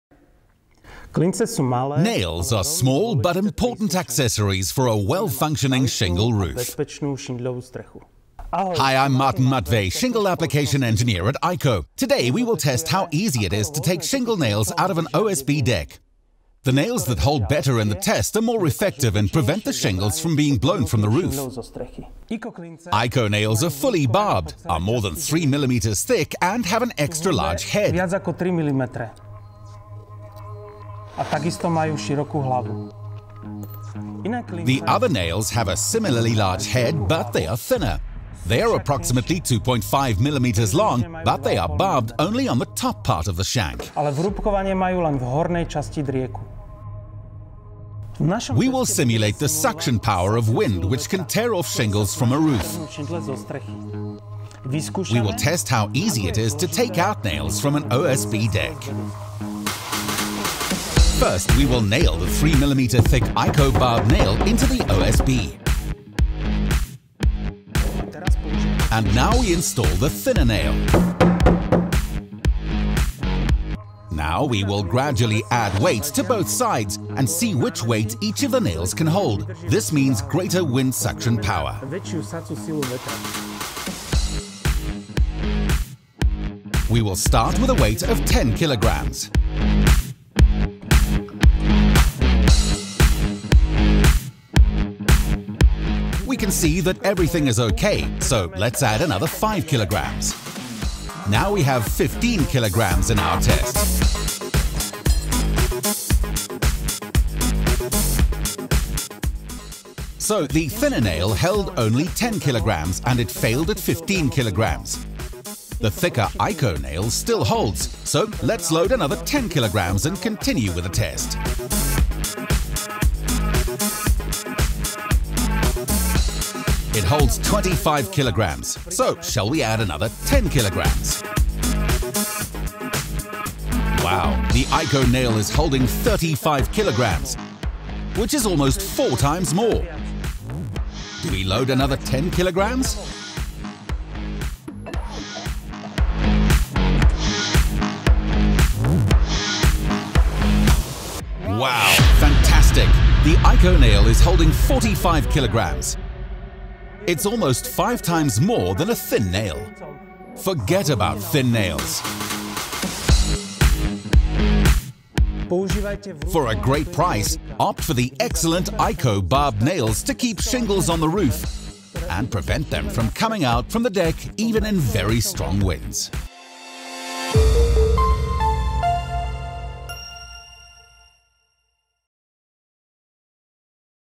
Test: How well do shingle nails hold into an OSB roof deck?
In this "Nail Pull Resistance Test" we will show you how easy or difficult it is to take shingles out of an OSB board. This will show how effective the nails are and which type of nails prevent the shingles from being blown from the roof.
Want to learn more about IKO nails?